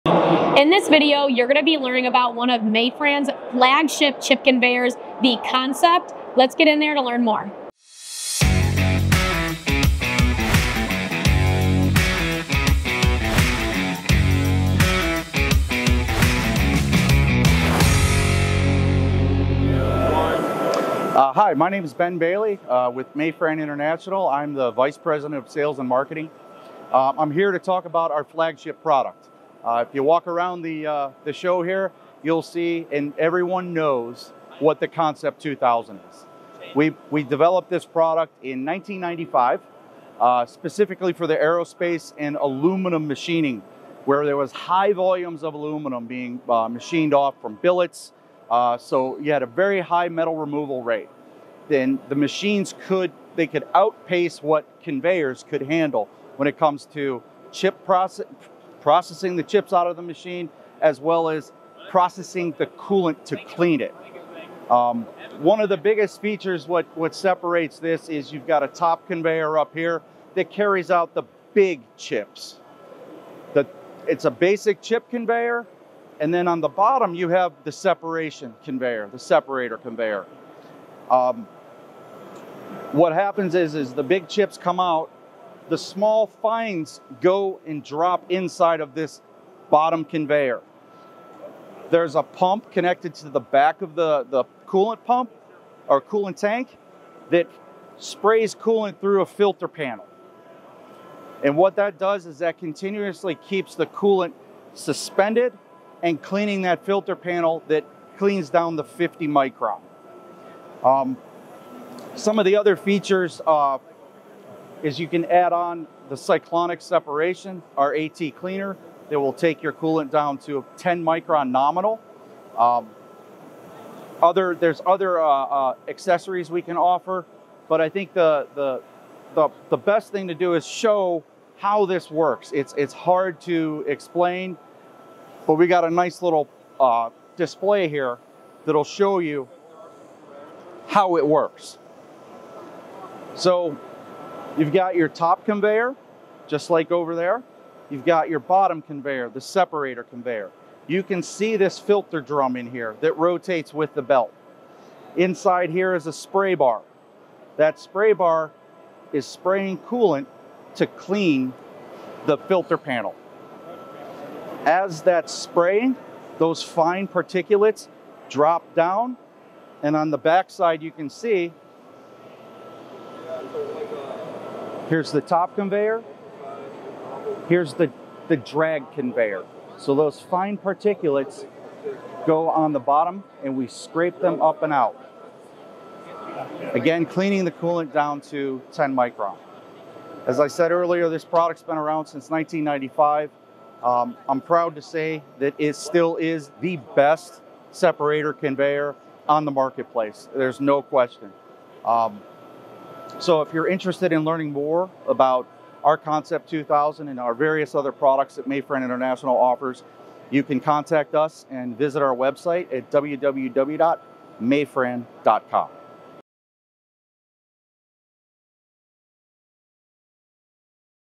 The flagship Consep 2,000 chip conveyor by Mayfran | IMTS 2024
Mayfran is well known for their Concep 2,000 chip conveyor, but why? Find out in this interview!

⁉️QUESTIONS – Leave them in the comments and we’ll try and get back to you as soon as possible!

In this video, Ben Bailey discusses Mayfran's flagship chip conveyor, the Consep 2000. Mayfran began manufacturing this conveyor in 1995, and it quickly became its flagship product, especially in the aerospace and aluminum machining space. The Consep became known for its ability to move chips and process coolant out quickly. All your large chips will get pushed out on the top conveyor belt, and the bottom conveyor will be your separator! Learn more about the Consep 2000 by watching this interview.